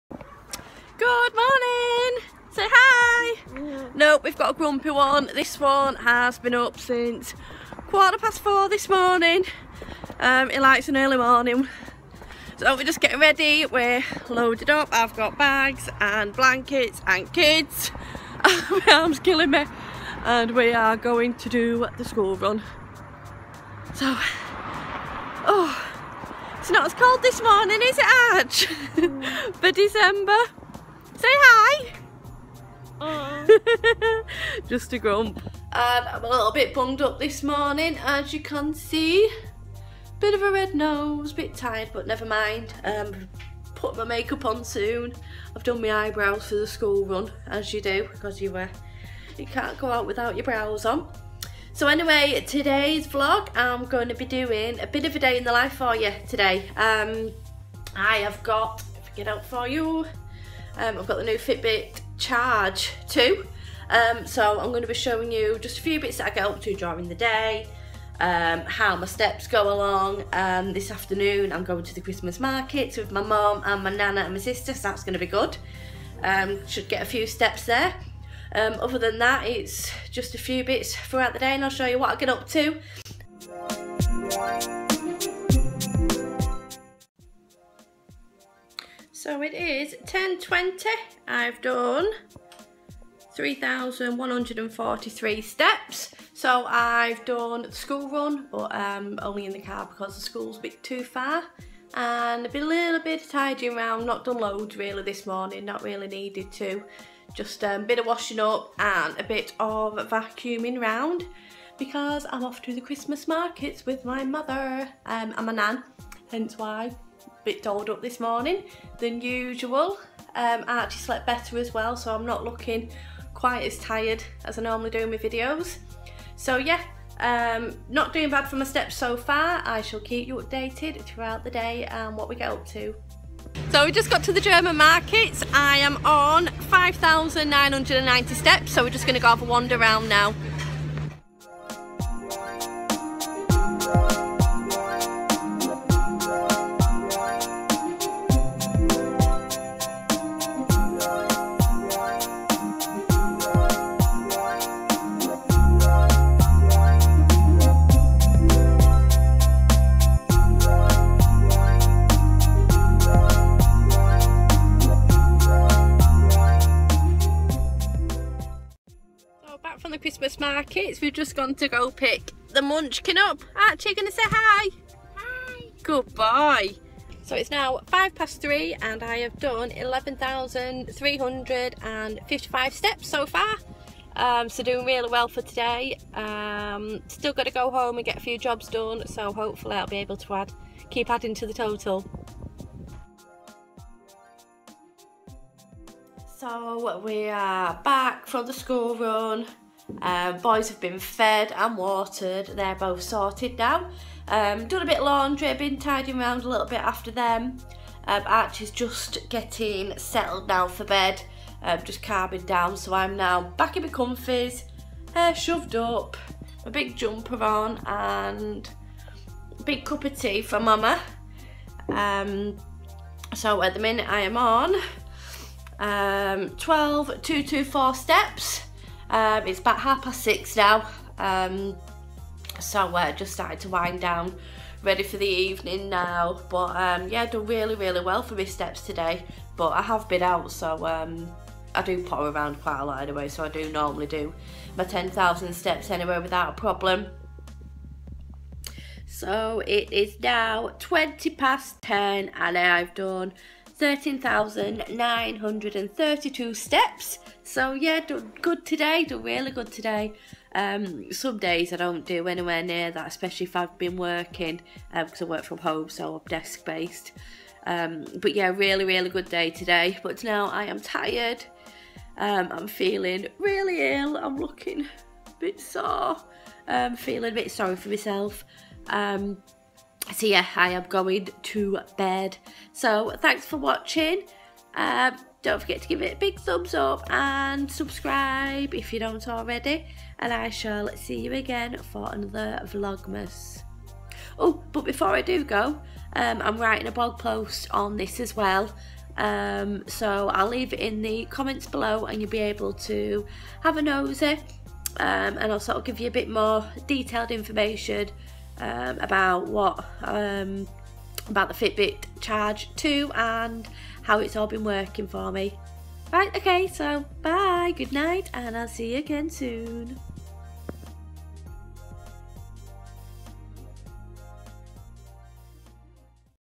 DITL WITH MY FITBIT CHARGE 2 | VLOGMAS DAY 13 - This Mama Vlogs
*OPEN ME* It's VLOGMAS Day 13 and it's a day in the life with my new FitBit Charge 2. Have a nosy at what I get up to and how many steps I take in a day

I'm so excited to be taking part this year, it's my first Christmas as a vlogger and I'm looking forward uploading a new video to my channel every evening at 6:30pm

FIND ME -